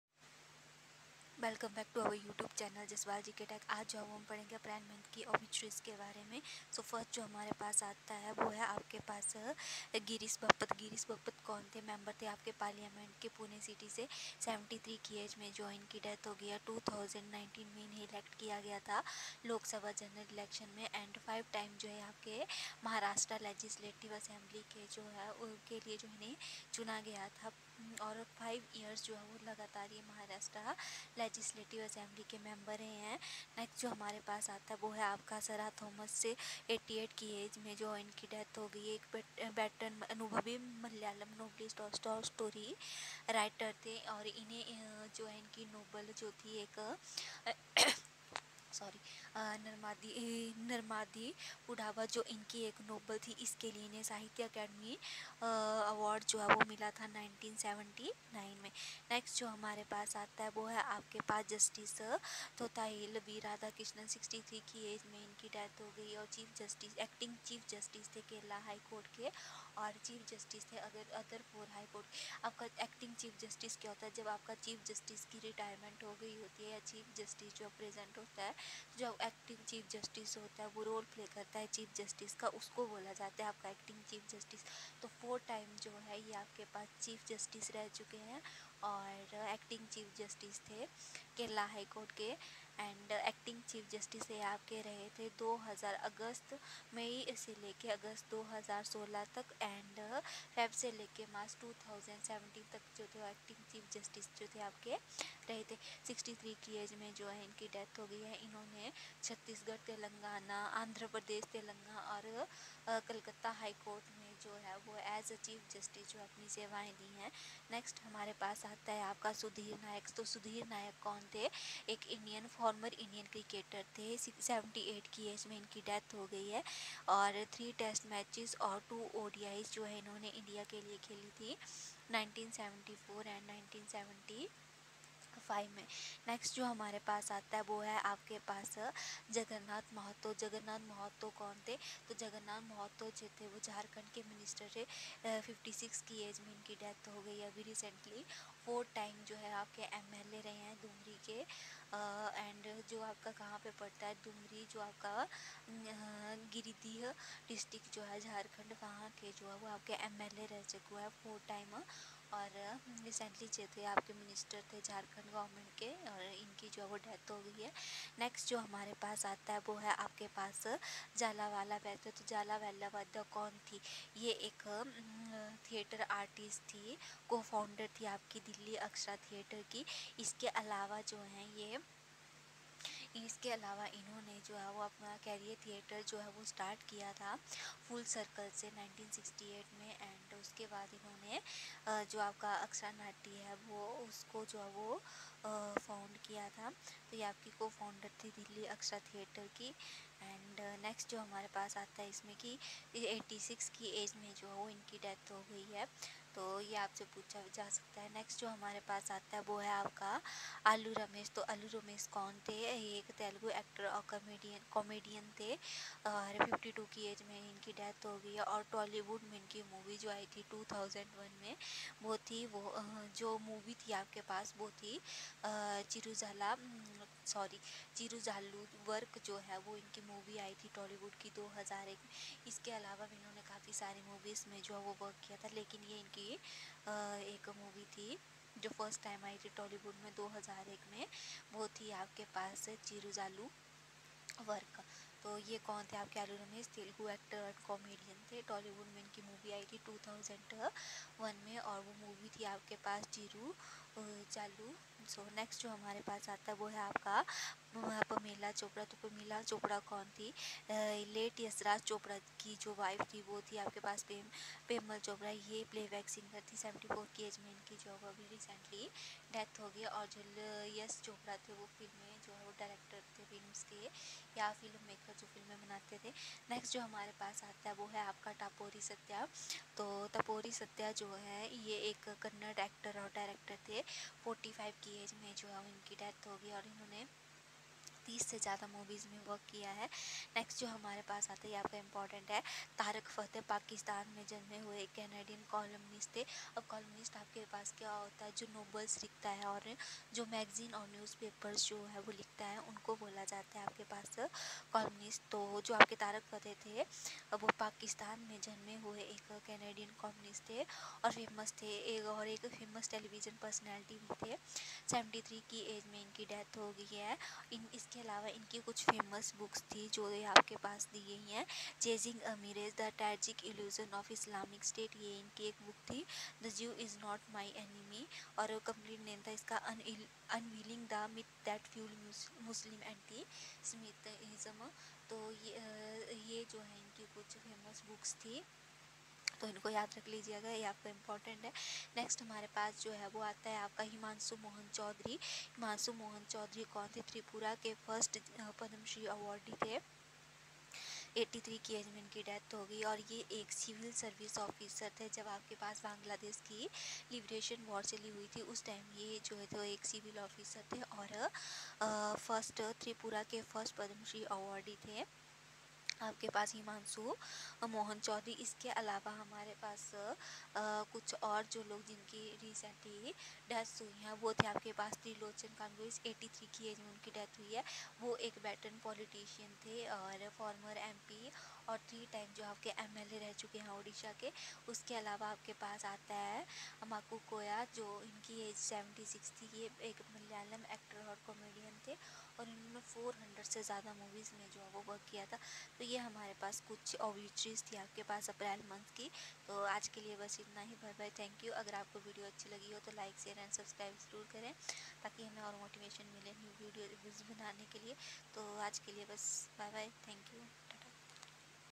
April month obutaries 2023 current affairs | deaths 2023
APRIL 2023 OBITUARIES FOR SSC CGL, CHSL MTS,state and central government exams Unveiling the Secret Obituaries
Current Affairs of the Past 6 Months
करेंट अफेयर्स obituaries  पिछले 6 महीने
 Important Obituaries  last 6 months for upcoming exam